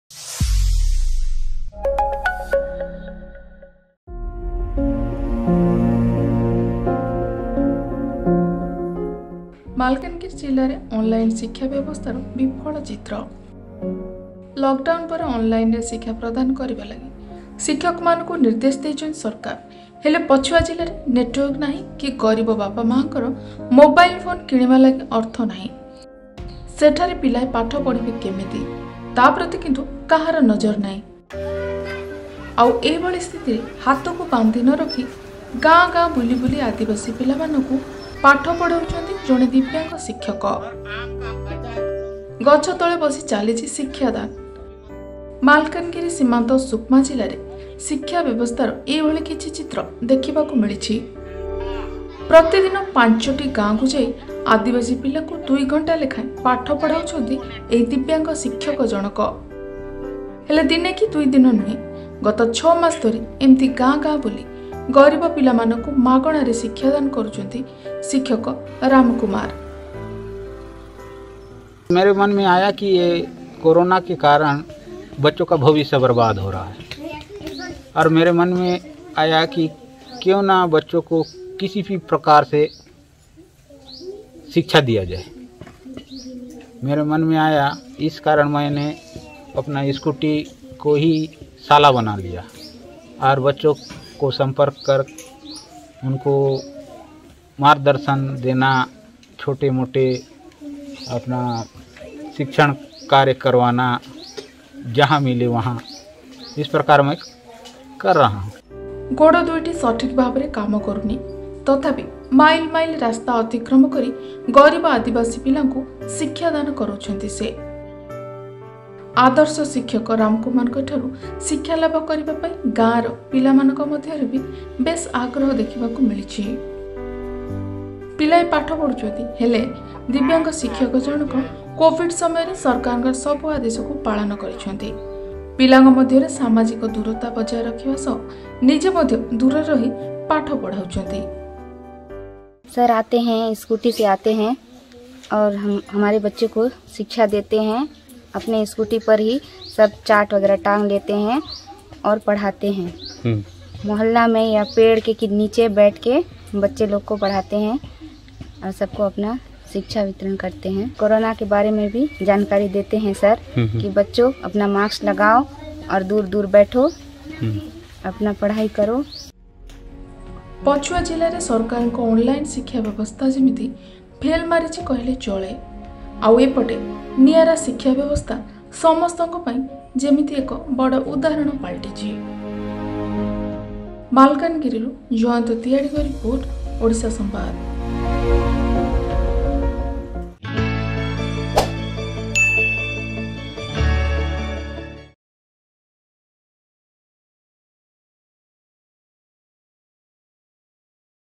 Disable Ram kumar Teaching in Backward Districts
ମାଲକାନଗିରି ଜିଲ୍ଲାରେ ଅନଲାଇନ୍ ଶିକ୍ଷା ବ୍ୟାବସ୍ତାର ବିଫଳ ଚିତ୍ର । ଲକ୍ ଡାଉନ୍ ପରେ ଅନ ଲାଇନରେ ଶିକ୍ଷା ପ୍ରଦାନ କରିବା ଲାଗି ଶିକ୍ଷକ ମାନଙ୍କୁ ନିର୍ଦ୍ଦେଶ ଦେଇଥିଲେ ସରକାର । ହେଲେ ପଛୁଆ ଜିଲ୍ଲାରେ  ପିଲାଏ ପାଠ ପଢିବେ କିପରି ତାହା କିନ୍ତୁ ଚିନ୍ତା କରିନାହାନ୍ତି ସରକାର । ଏଭଳି ସ୍ଥିତିରେ ହାତକୁ ବାନ୍ଧି ନରଖି ଗାଁ ଗାଁରେ ଯାଇ ଆଦିବାସି ପିଲାଙ୍କୁ ଗତ ଛଅ ମାସ ଧରି ପାଠ ପଢାଉଛନ୍ତି ଜଣେ ଦିବ୍ୟାଙ୍ଗ ଶିକ୍ଷକ । ଗାଁ ଗାଁ ବୁଲି ଗରିବ ଆଦିବାସି ପିଲାଙ୍କୁ ମାଗଣାରେ ଶିକ୍ଷାଦାନ କରୁଛନ୍ତି ଦିବ୍ୟାଙ୍ଗ ଶିକ୍ଷକ ରାମ କୁମାର । ଦେଖିବା ଓଡିଶା ସମ୍ବାଦର ଏକ୍ସକ୍ଲୁସିଭ ରିପୋର୍ଟ